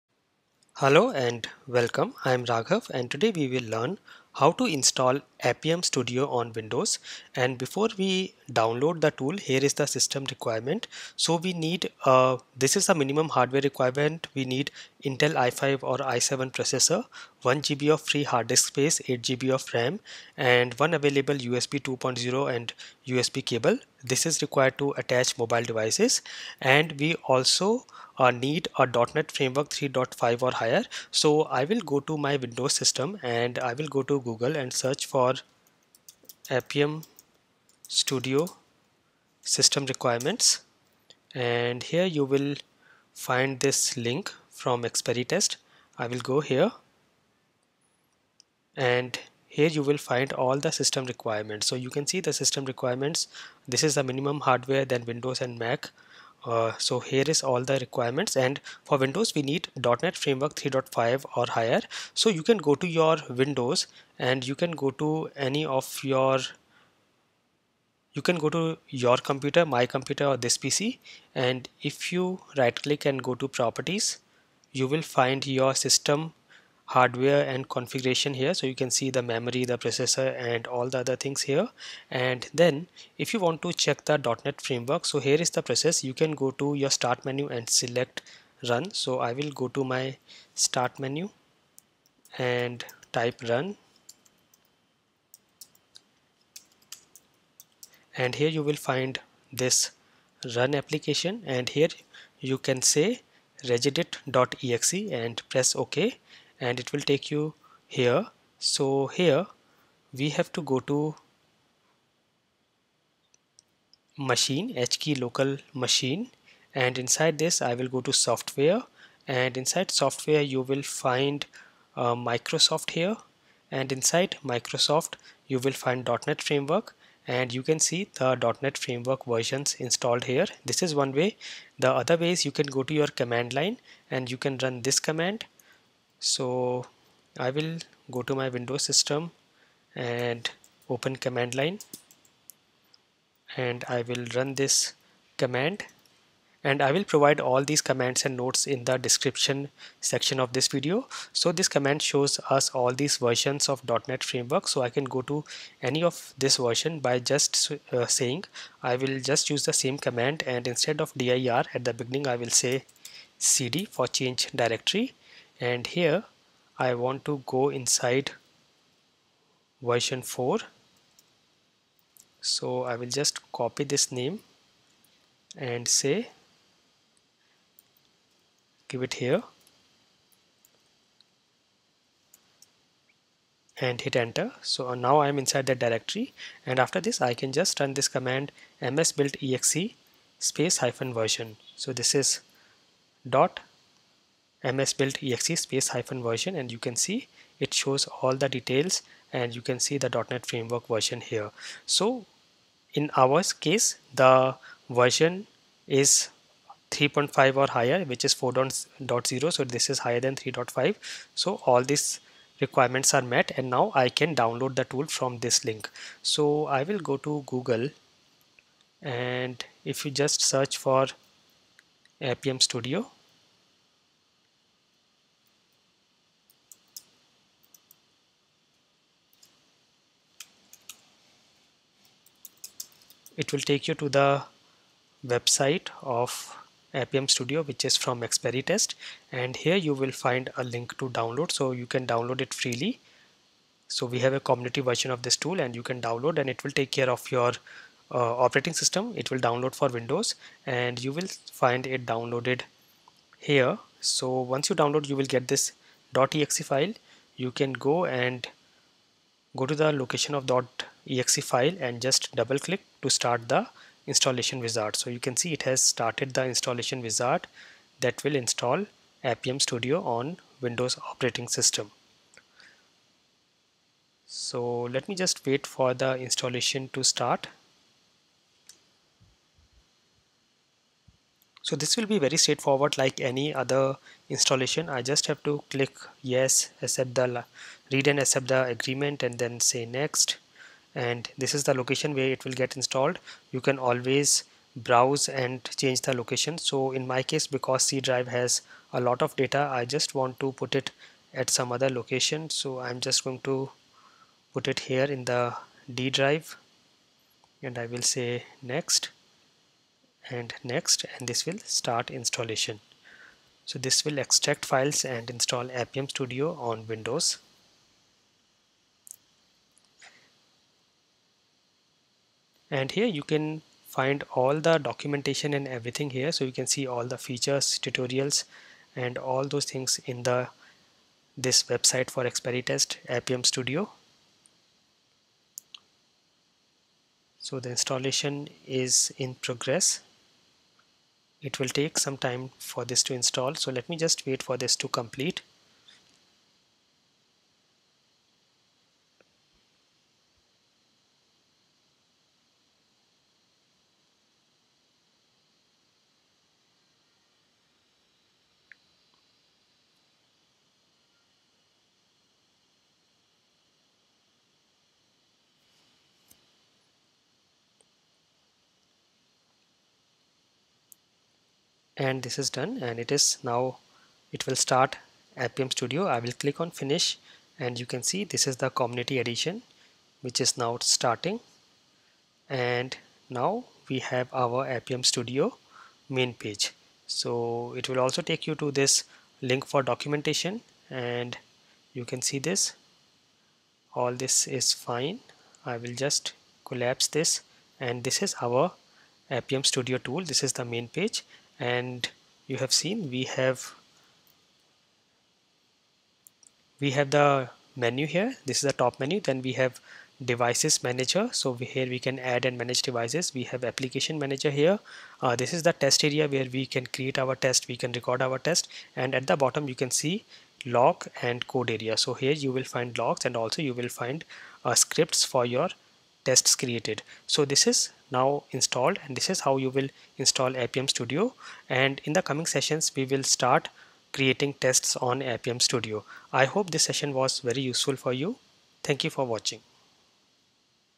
Appium Beginner Tutorial 12 | How to Install Appium Studio on Windows
Appium Studio
IDE for appium automation
Enhanced ver of open source appium with a GUI and features
Can develop, execute tests and view test results
Uses Appium/Selenium webdriver api
Created by experitest

Raghav